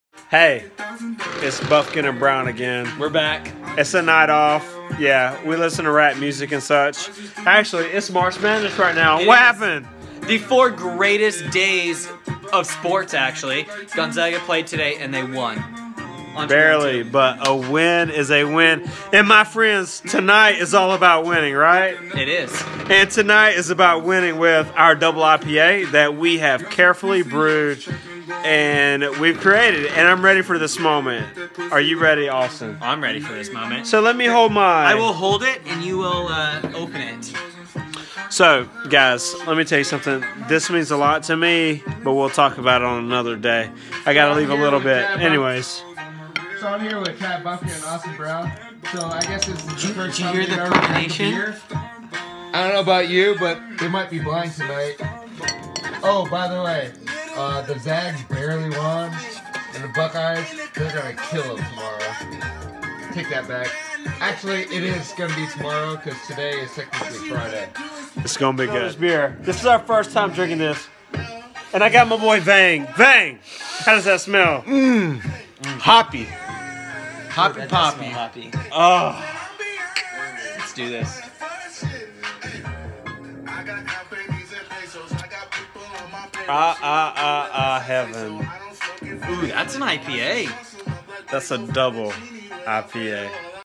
Brew 1 Episode 11- Feels like the first time
Season Finale of Brew 1- The very first taste of Brewer’s Best DIPA with our guest star, Dr. Andrew Vang making a highly anticipated special appearance